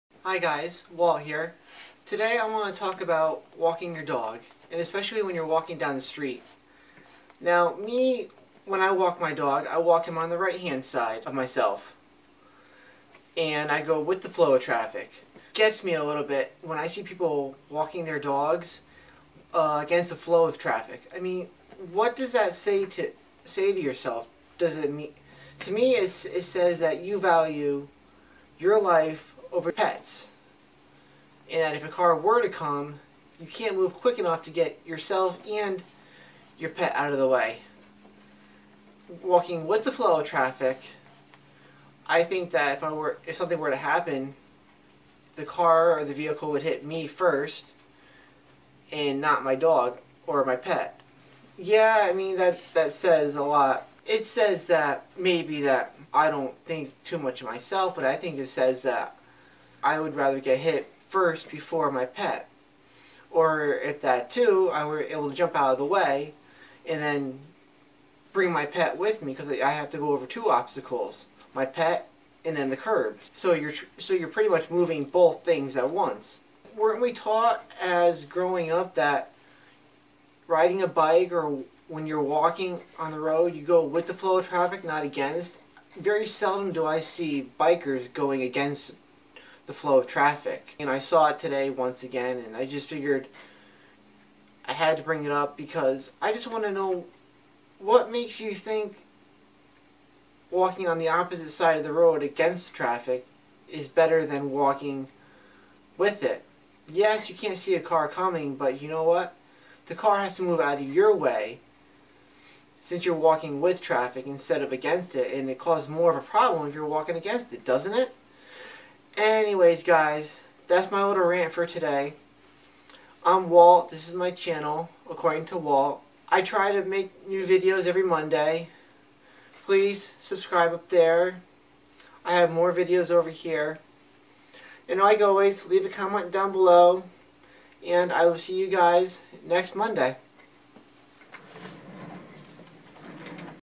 Walking a dog AccordingtoWalt
Have you seen this when you're out in the car or walking your own dog, there is a person walking on the opposite side of the road going against the flow of traffic. Or, how about when they put there dog on the outside of themselves, To me it just says that somewhere it that person's mind they don't value their pet's safety as high as their own. It also makes the cars that have to get around that person easier if they are moving with the flow. I just want to know that if you walk or ride a bike with the flow of traffic why would a person chose to go against it when they are out walking their pet? Let me know what you furry bunnies think about all this. See you next Monday (or not, but most likely)